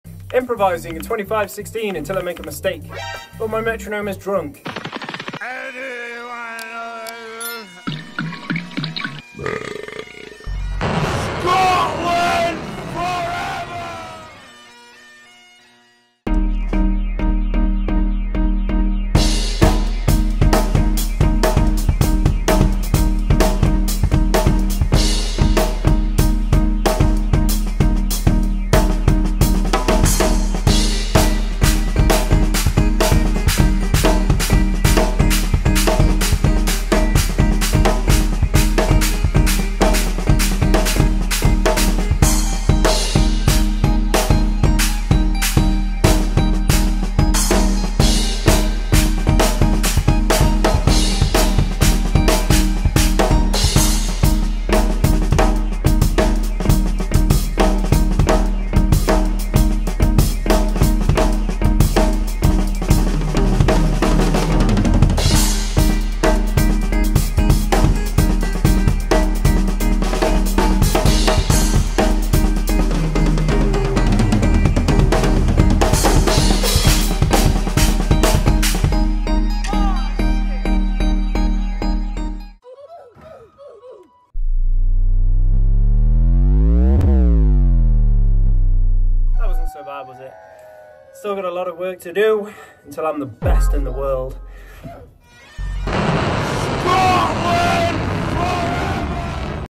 improv in 25/16 until i fail but my metronome is drunk
0:00 intro
0:16 drum solo until i fail
1:18 i made a mistake 😭
1:23 Deactivate Domain Expansion
1:29 Scotland Foreverrrrrr!